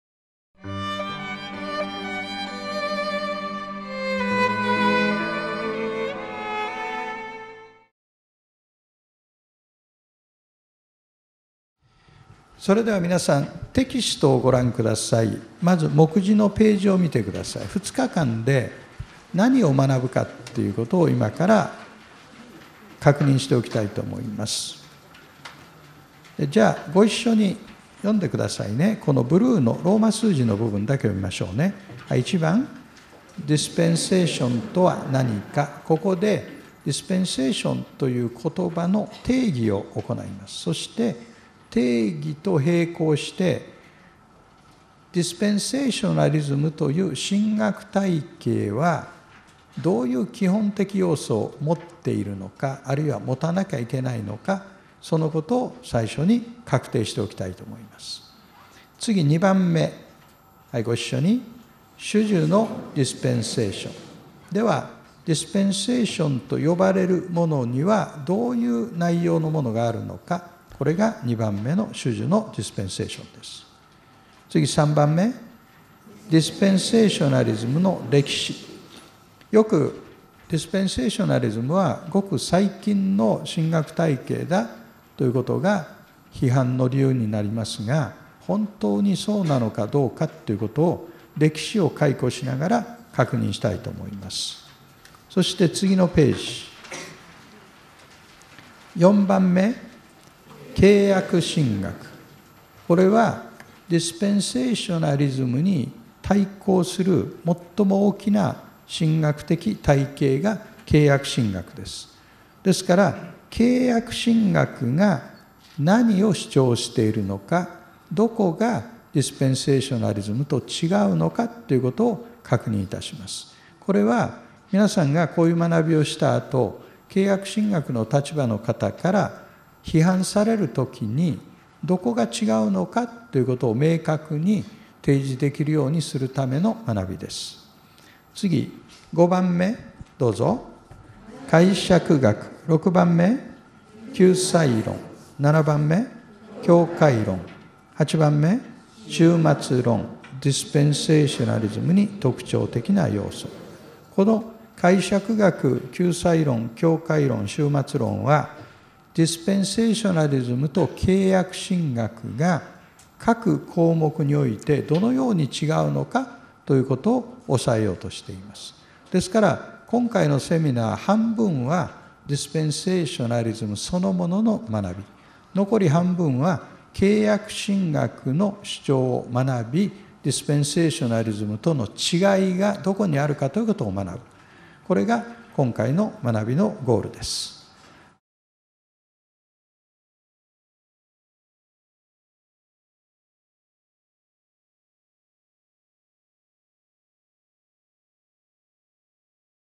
『ディスペンセーショナリズムとは何か』PRビデオ 2016フルクテンバウム博士セミナー
ディスペンセーショナリズムとは何か
―体系的な聖書理解を求めて―
2016 年フルクテンバウム博士セミナー
講師：アーノルド・フルクテンバウム博士
通訳：中川健一

2000年より始まった恒例のフルクテンバウム・セミナーも、ついに最終回を迎えます。
総まとめのセミナーでは、以下のような内容を学びます。
＊体系的に聖書を理解するためには、何を知っておく必要があるか。
＊聖句を適用する際に注意すべき点は何か。
＊ディスペンセーションとはどういうものか。
＊ディスペンセーショナリズム批判にどのように答えたらよいのか。

運営団体：ハーベスト・タイム・ミニストリーズ